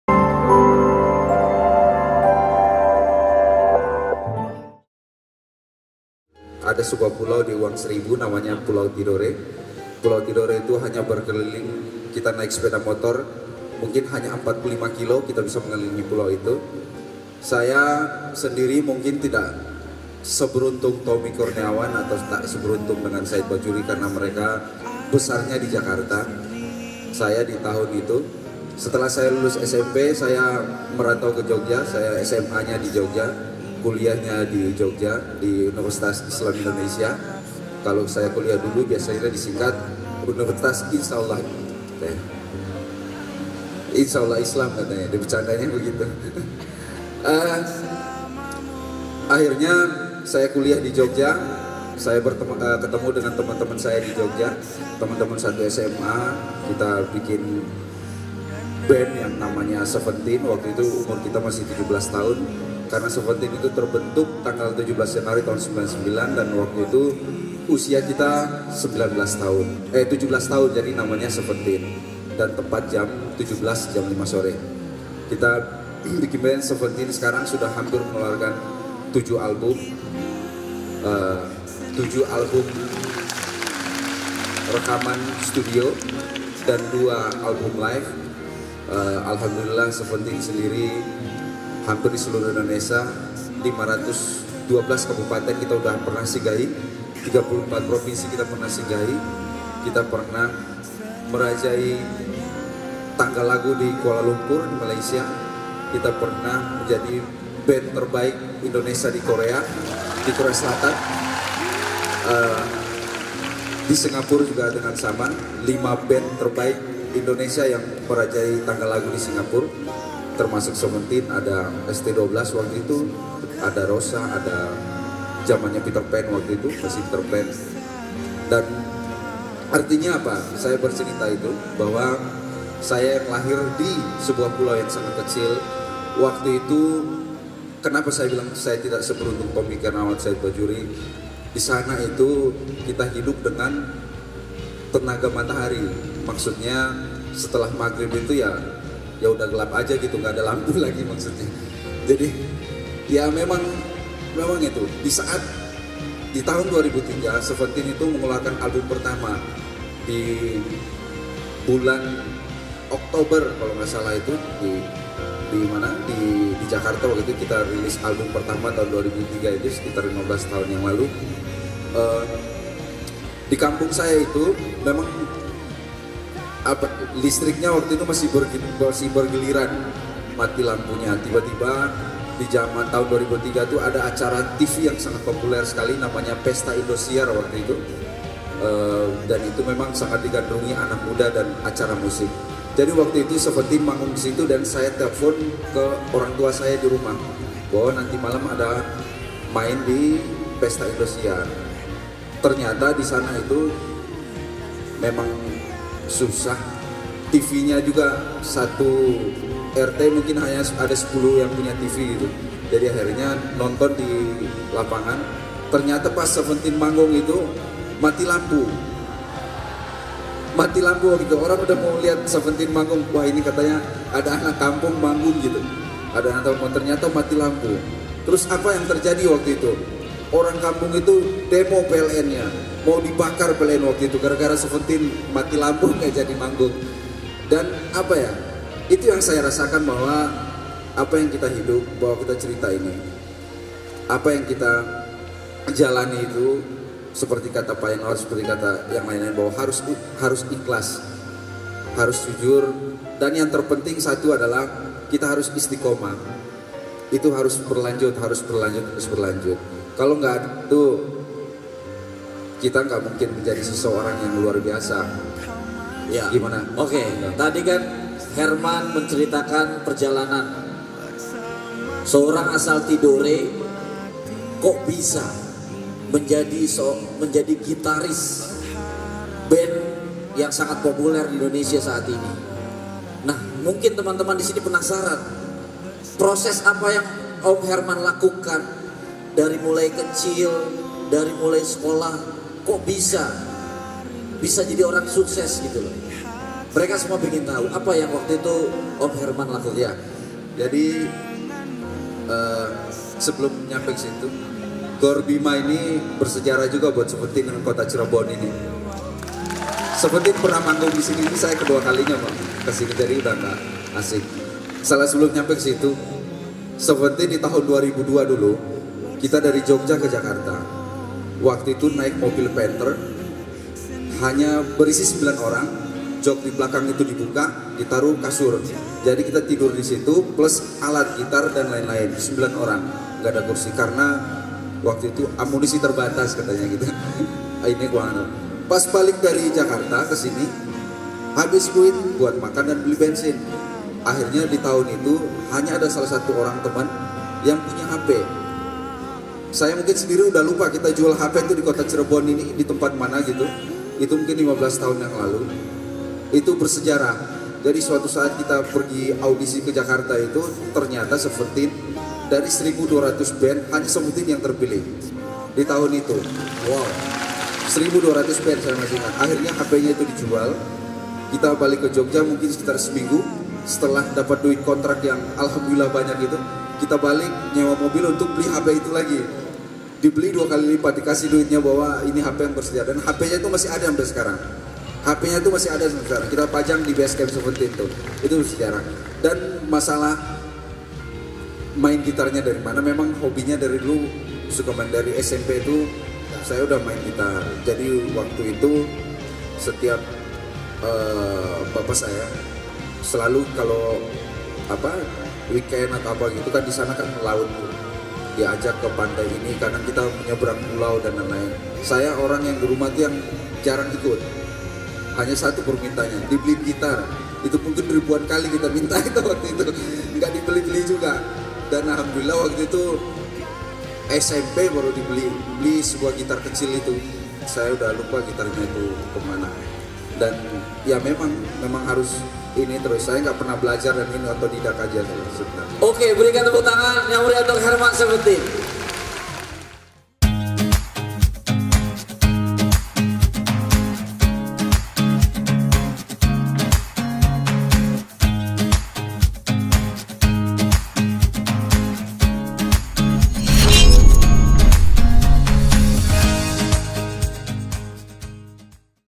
Mengharukan, Herman Seventeen Cerita Perjalanan Kesuksesan Band Seventeen
Ingin tau lebih detail mengenai Training Sukses Itu Mudah dan Cara Mudah Menang Pemilu?